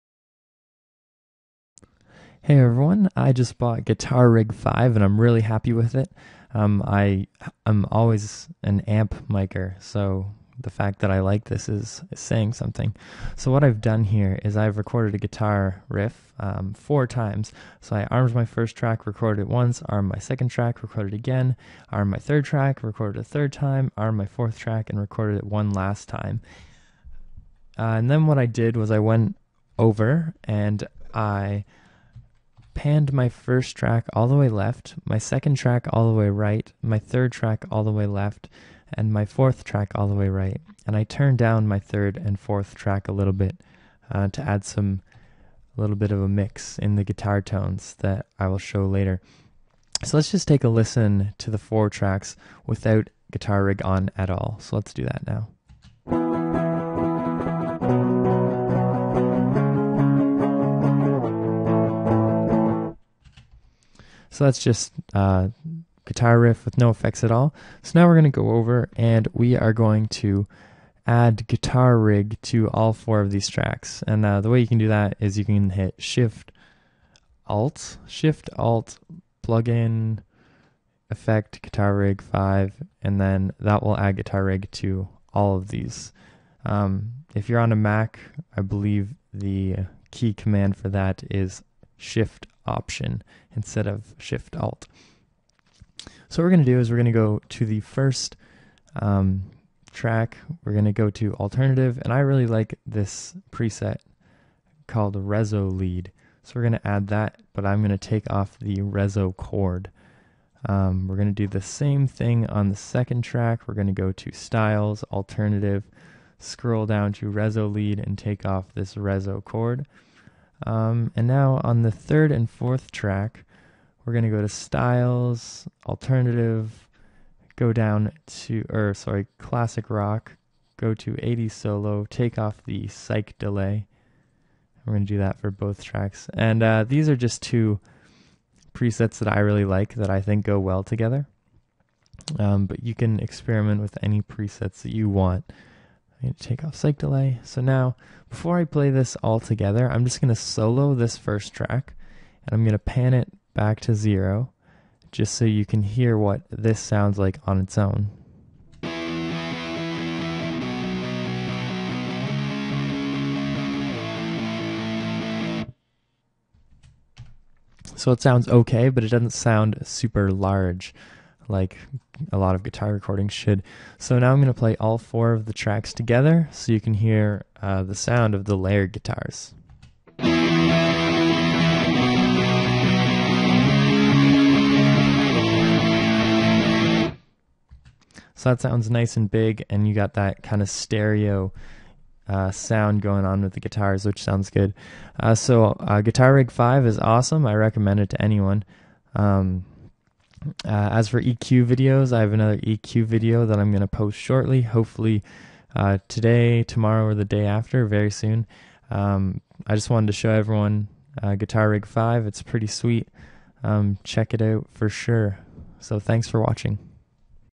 Layering With Guitar Rig 5
In this video I demonstrate layering with Guitar Rig 5 to get a big, stereo guitar sound. You can get some amazing sounds out of Guitar Rig and I'm very impressed! It is definitely worth the small investment.